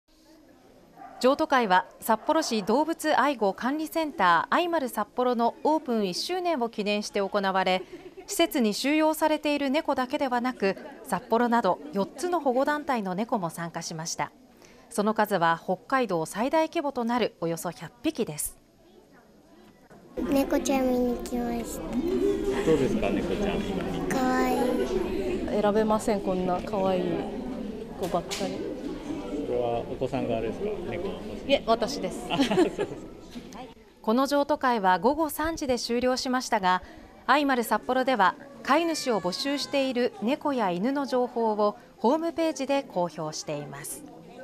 北海道最大級のネコの譲渡会 ４団体＋あいまるさっぽろ 約１００匹新たな飼い主を待つ
北海道最大級のネコの譲渡会です。札幌市中央区の「あいまるさっぽろ」でおよそ１００匹のネコたちが新たな飼い主を待っています。

譲渡会は札幌市動物愛護管理センター「あいまるさっぽろ」のオープン１周年を記念して行われ、施設に収容されているネコだけではなく、札幌など４つの保護団体のネコも参加しました。

その数は北海道最大規模となるおよそ１００匹です。

女の子「ネコちゃん見に来ました（どうですか？）かわいい」
母親「選べません、こんなにかわいいのばかりで（お子さんが？）いえ、私が」

この譲渡会は、９日午後３時で終了しましたが、「あいまるさっぽろ」では飼い主を募集しているネコやイヌの情報をホームページで公表しています。